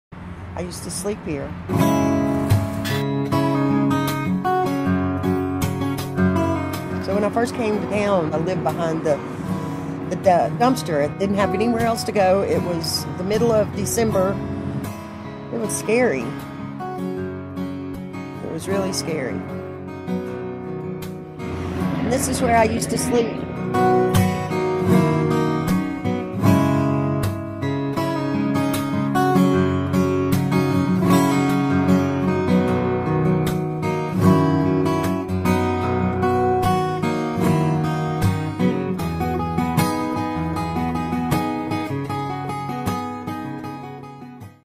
I Used to Sleep Here
Sunni shares her story of  having to sleep behind the shelter by a dumpster because there was no room in the local shelters.